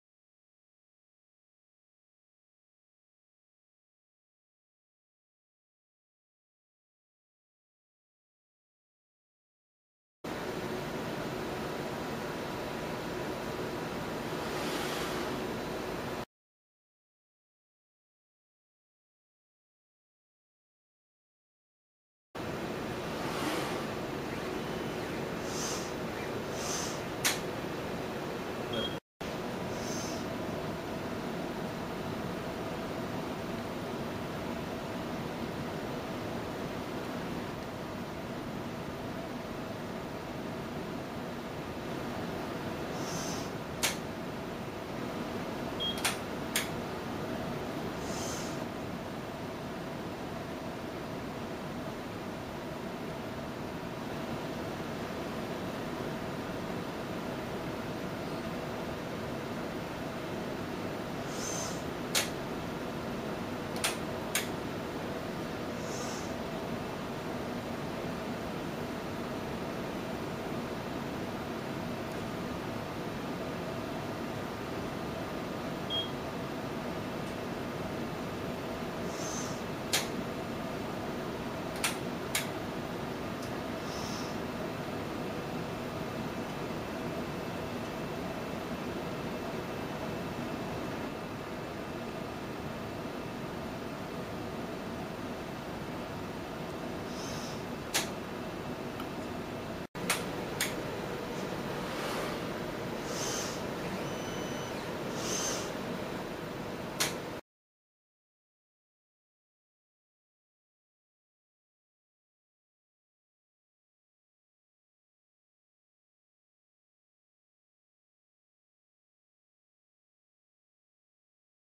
PickGel- Gel Extraction add-on for Tecan robot
Comming soon from SciRobotics ...The PickGel add-on for Tecan robots.
PickGel enable robotics gel extraction of bands that  is integrated ona Tecan robot and can continue to downstream processing by the robot.